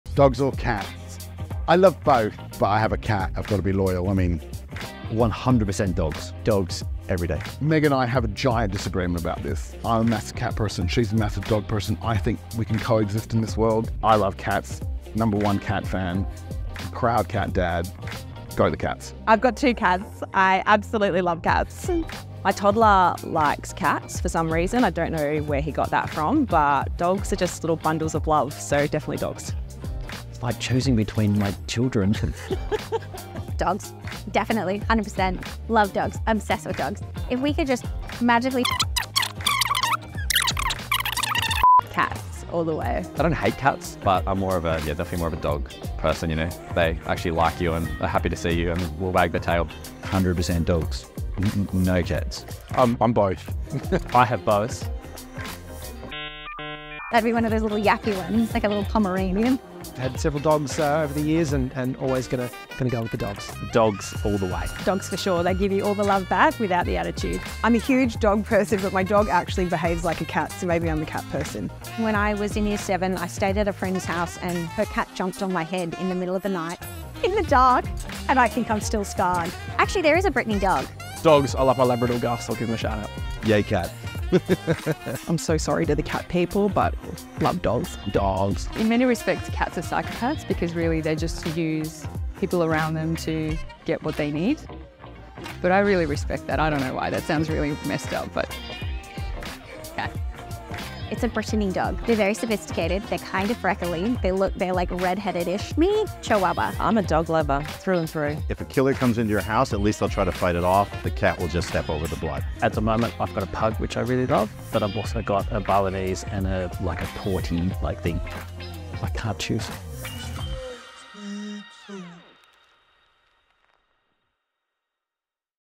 Dogs or cats? 🤔
The most pressing debate at StateofSocial23? Simple, 🐕 vs 🐈.